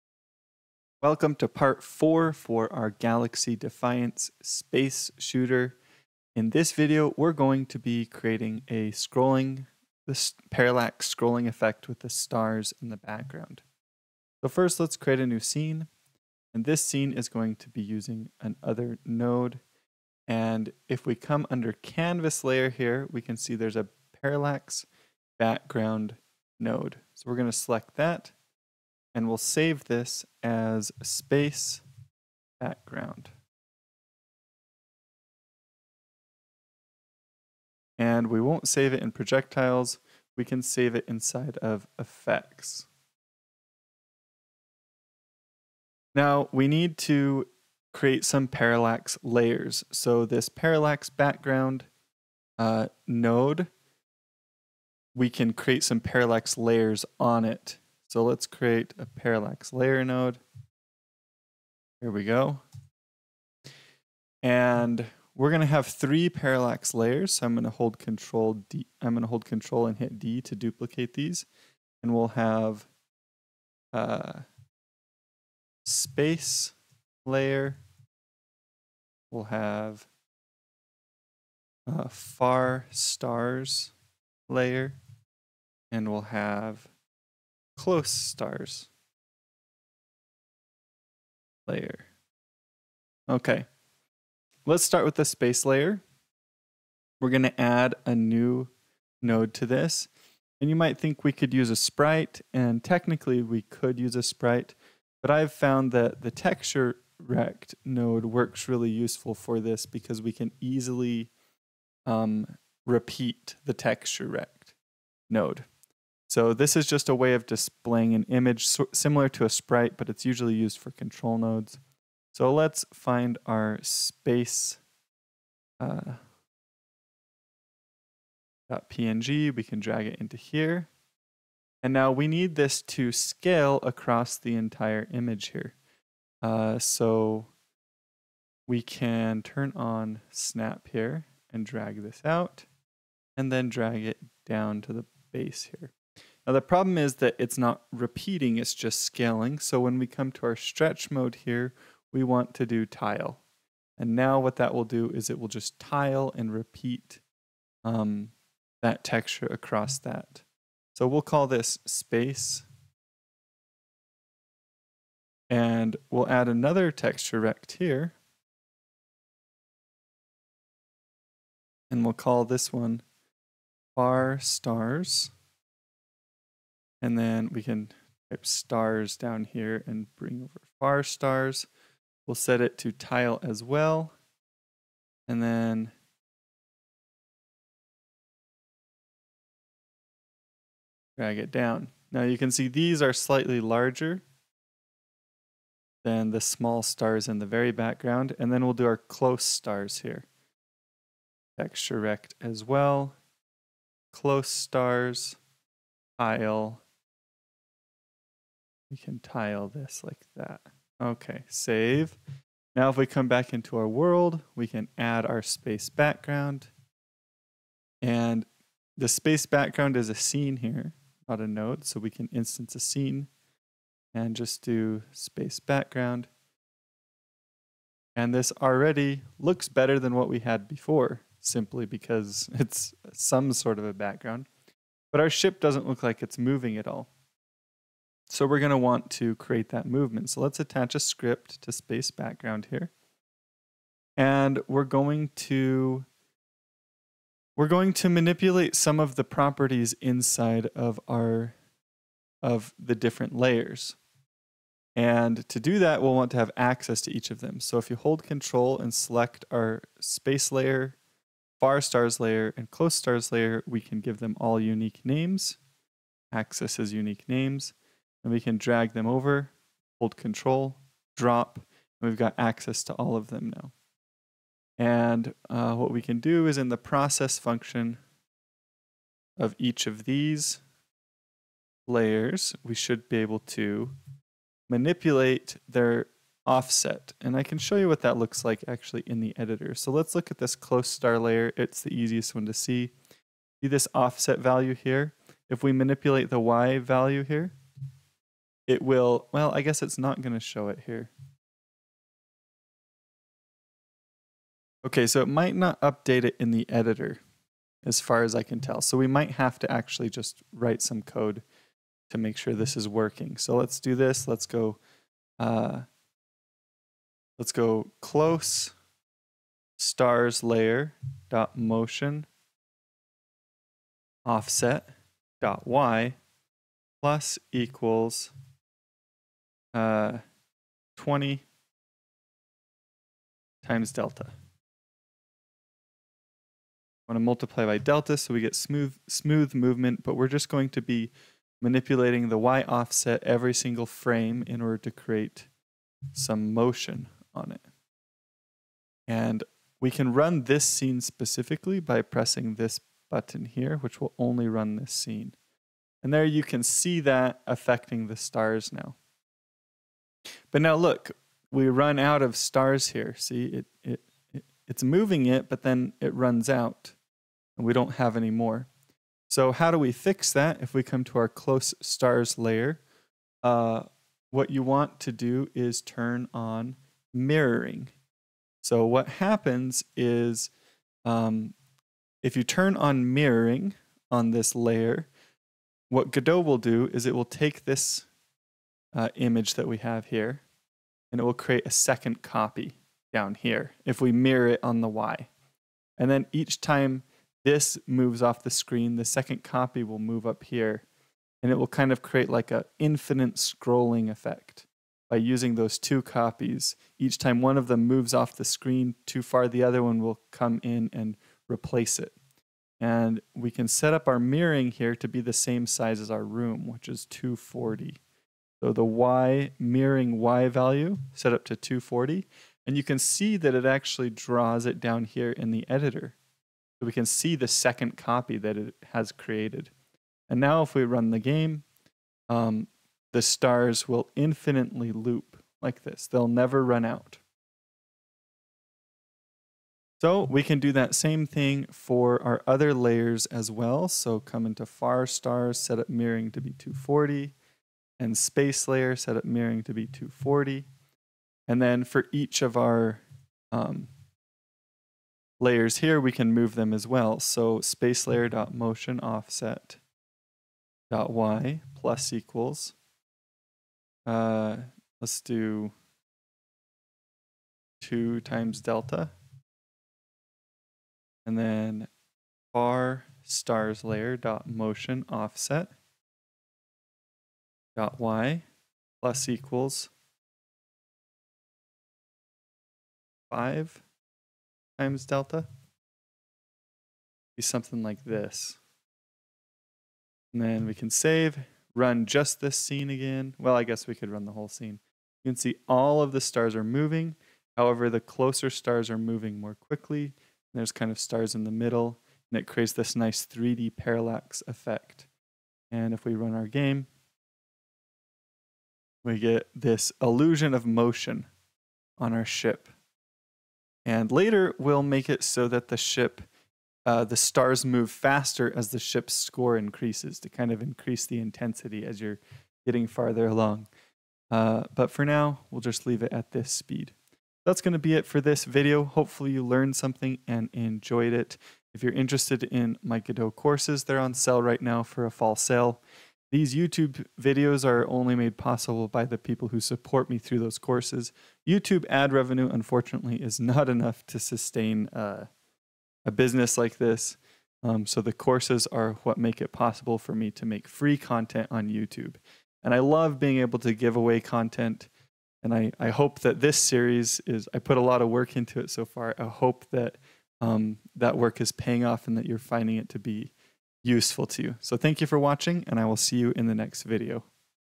Godot 4 Tutorial - Space Shooter Using Components - P4 Scrolling Background
If you are interested in taking a deeper dive into the Godot game engine you can buy my 1-bit Godot Course at this

Here is the GitHub repo for this project (updated as I record videos)